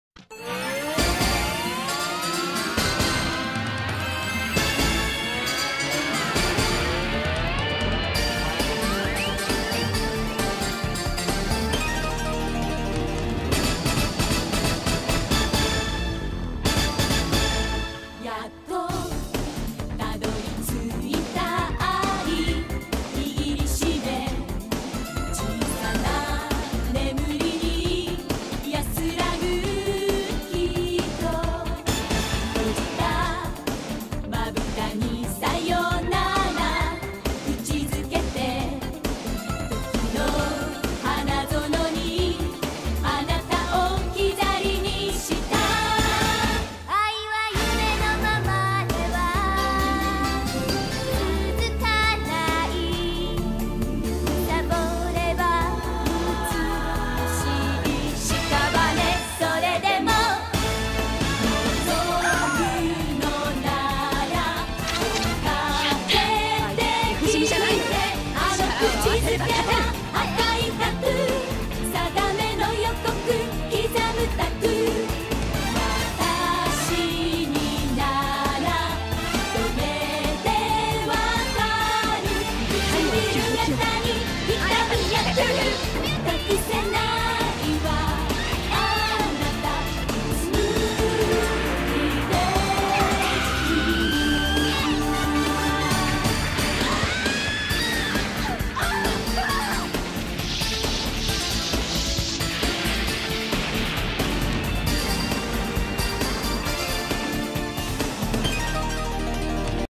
Battle dome Starlight vs Venus
This is my attempt at a battle between the Starlights and Venus. Enjoy!